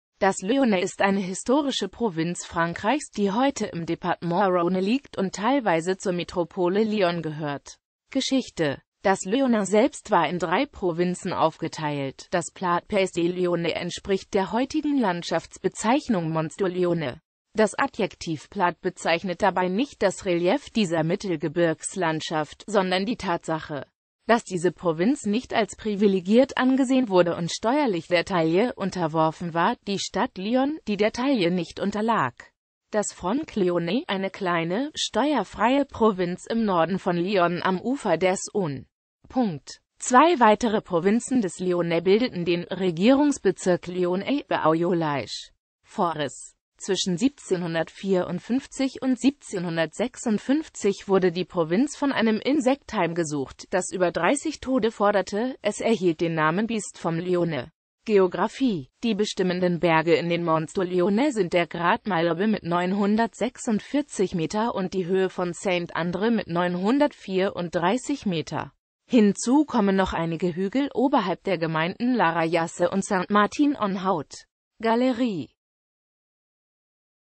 Lyonnais
Vielen Dank für Ihre Unterstützung:

Das Lyonnais ist eine historische Provinz Frankreichs, die heute im Département Rhône liegt und teilweise zur Metropole Lyon gehört.

Bild-Copyright-Informationen
Urheber Info: Syryatsu